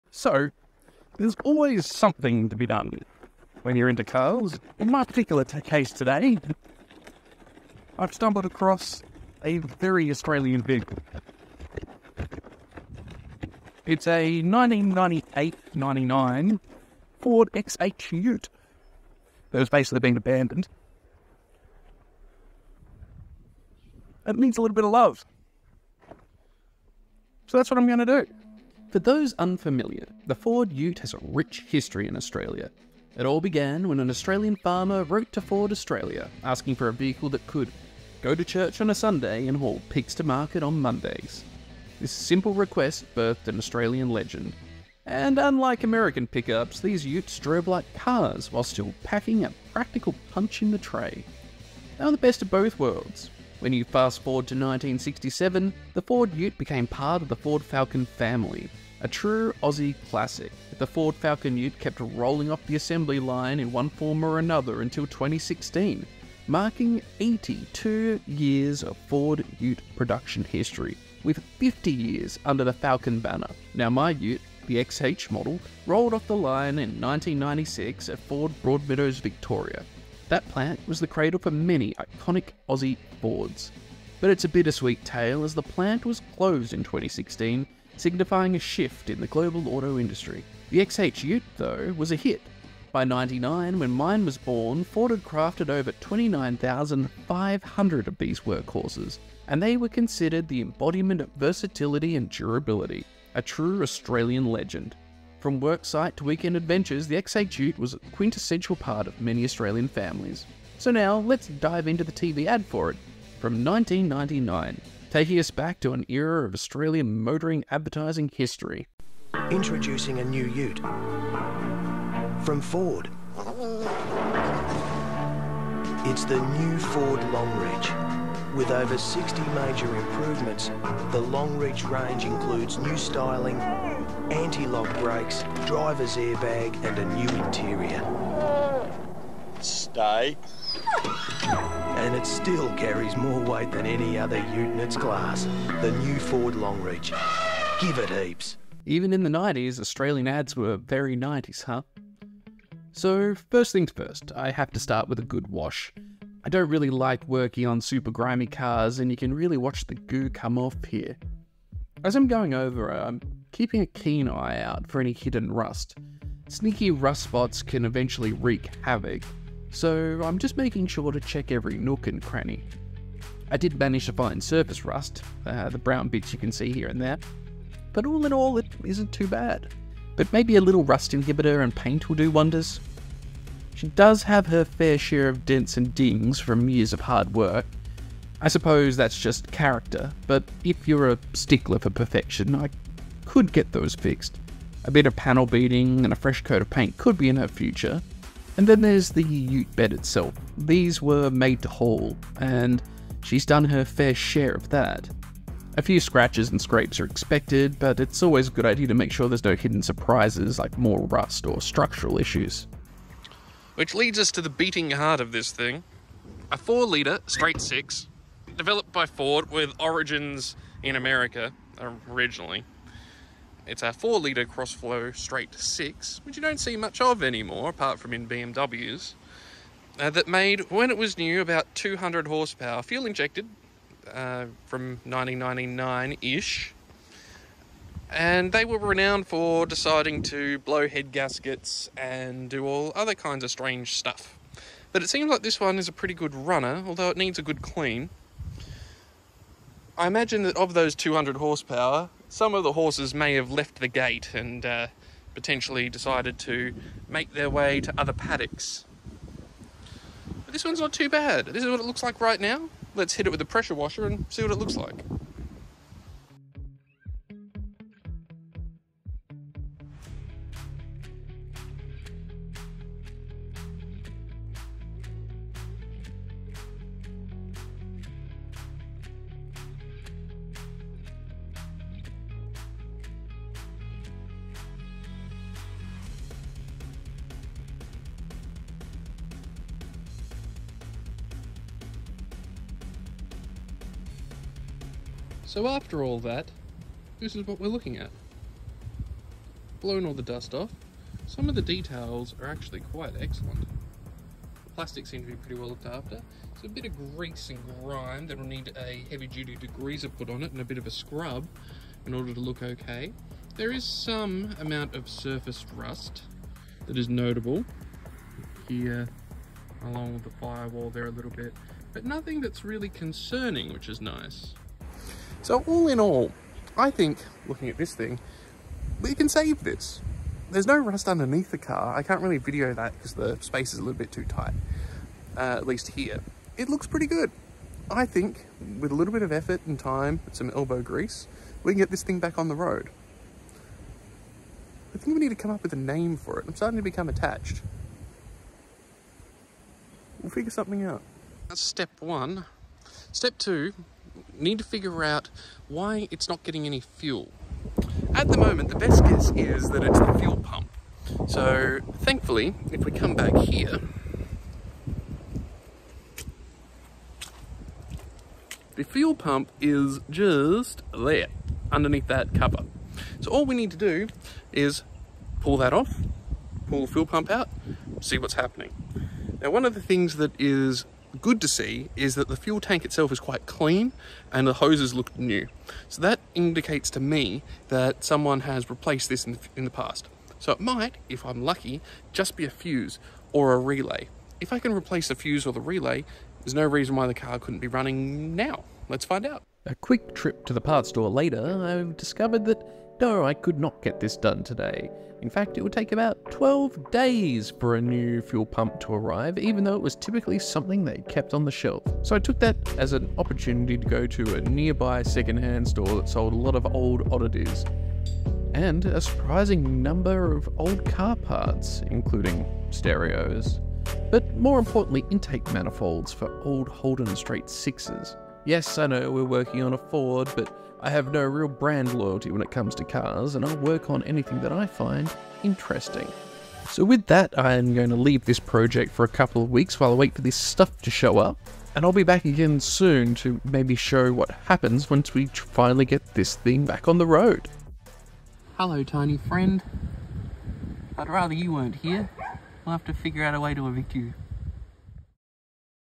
Unearthing Aussie Auto History: Ford Ute Chronicles | October VLOG
🚗 Discover the LEGENDARY Ford Ute! 🇦🇺

Join me on a Thrilling Journey Through Aussie Automotive History! We're diving into the  history of the Ford Ute in Australia! From an Aussie farmer's letter to automotive legend, we're exploring how this iconic vehicle became a national treasure.

Buckle up as we take an up-close look at my own Ford Falcon XH Ute, sharing the thrills of owning a piece of history and our efforts to restore it.
Get ready for a nostalgic ride as we revisit the TV ads of 1999, giving you a taste of what motoring was like back then.

We'll show you the start of our restoration process, what to look for when bringing these classics back to life, and how we're making it roadworthy once again.